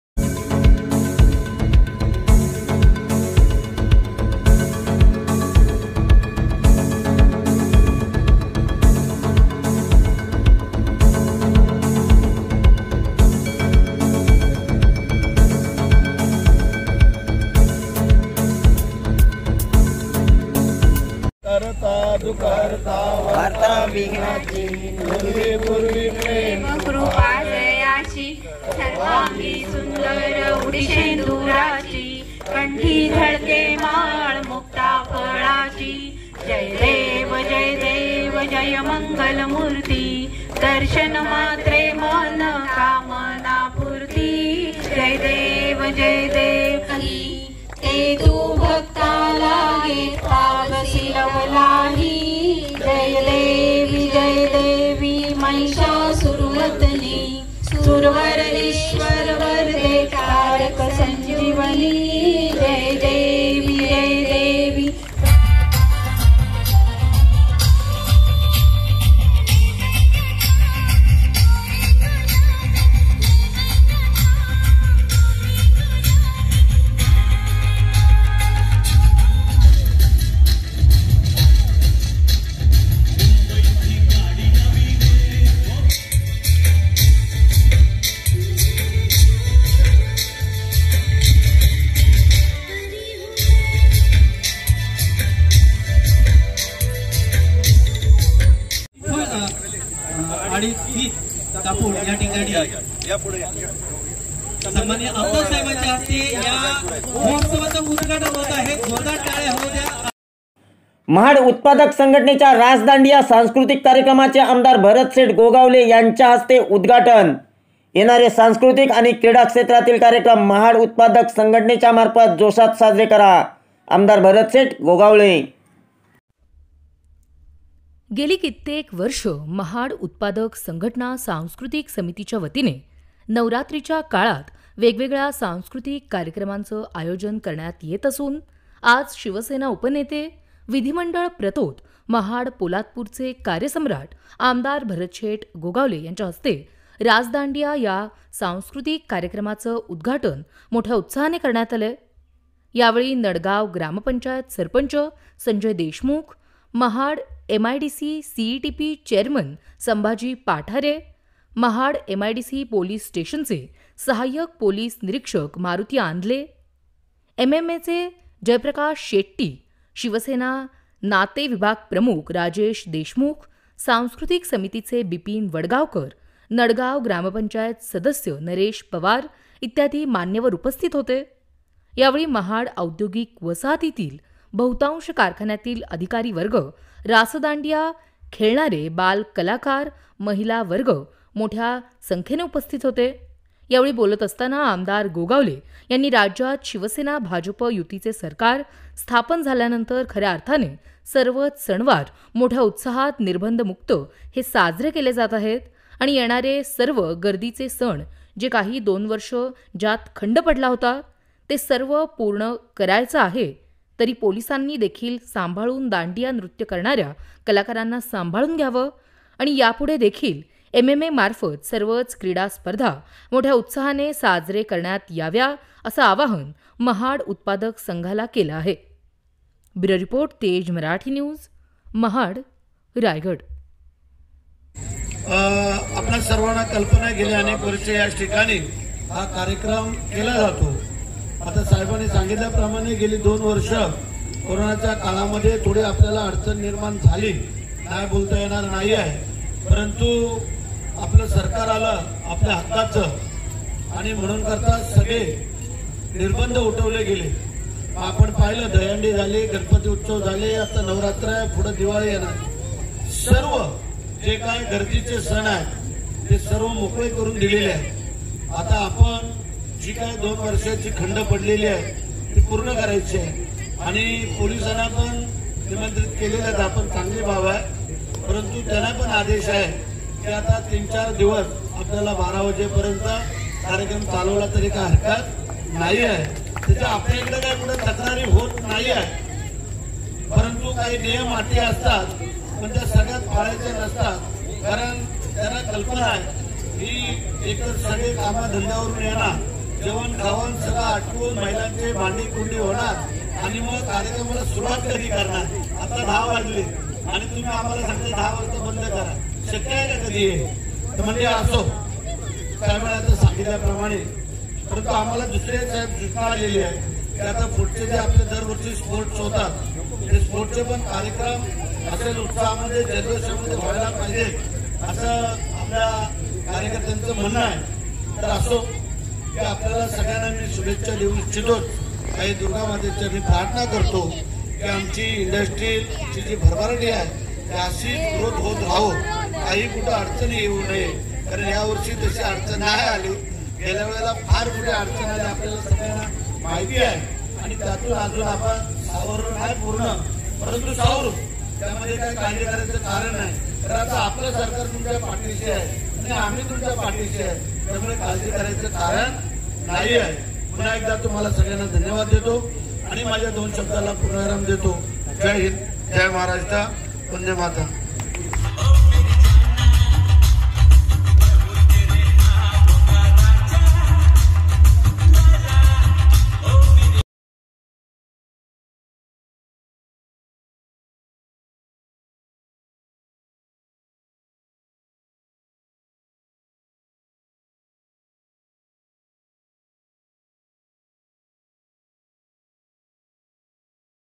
महाड उत्तपादक संघटनेच्या रासदांडिया सांस्कृतीक कार्यक्रमाचे आ.भरतशेट गोगावले यांच्या हस्ते उद्घाटन
ब्रेकिंग न्यूज

महाड उत्तपादक संघटनेच्या रासदांडिया सांस्कृतीक कार्यक्रमाचे आमदार भरतशेट गोगावले यांच्या हस्ते उद्घाटन

येणारे सांस्कृतीक आणि क्रिडा क्षेत्रातील कार्यक्रम महाड उत्तपादक संघटनेच्या मार्फत जोसात साजरे करा: आमदर भरतशेट गोगावले

अँकर:- गेली कित्तेक वर्ष महाड उत्पादक संघटना सांस्कृतीक समितीच्या वतीने नवरात्र काळत वेगवेगळ्या सांस्कृतीक कार्यक्रमांचे आयोजन करत असून आज शिवसेना उपनेते विधीमंडळ प्रतोद महाड पोलादपूरचे कार्यसम्राट आमदार भरतशेट गोगावले यांच्या हस्ते रासदांडिया या सांकृतिक कार्यक्रमाचे उद्घाटक मोठ्या उत्साहाने करण्यात आले यावेळी नडगाव ग्रामपंचायत सरपंच संजय देशमूख, महाड एम एम ए सि ई टी पी चेरमन संभाजी पाठारे, महाड एम आय डी सी पोलिस स्टेशनचे सहाय्यक पोलीस निरीक्षक मारूती आंधले, एम एम ए चे जयप्रकाश शेट्टी, शिवसेना नाते विभागप्रमुख राजेश देशमुख सांकृतीक समितीचे बिपीन वडगावकर नगडगाव ग्रामपंचायत सदस्य नरेश पवार इत्यादी मान्यवर उपस्थित होते यावेळी महाड औद्योगिक वसाहतीतील बहुतांशी कारखखान्यातील अधिकारी वर्ग रासदांडिया खेळणारे बाळ कलाकर महिला वर्ग मोठ्यासंख्येन उपस्थित होते

यावेळी बोळत असताना आमदार गोगावले यांनी राज्यात शिवसेना भाजपा युतीचे सरकार स्थापन झाल्यानंतर खर्या आर्थाने सर्वच सनवार मोट्या उत्साहात निर्बंध मुक्त साजरे केले जात आहेत आणि येणारे सर्व गर्दीचे सन जि काही दोन वर्ष खंड पडली आहे ती सर्व पुर्ण करायची आहे तरी पोलिसांनी देखिल सांभाळून दांडीया नृत्य करणार्या कलाकारांना सांभाळून घ्यावे आणि यापुढील होणारे एम एम ए मार्फत सर्वच क्रिडा स्पर्धा मोट्या उत्साहात साजरे करावे असे आवाहन महाड उत्पादक संघाला केले आहे

ब्यूरो रिपोर्ट तेज मराठी न्यूज महाड रायगड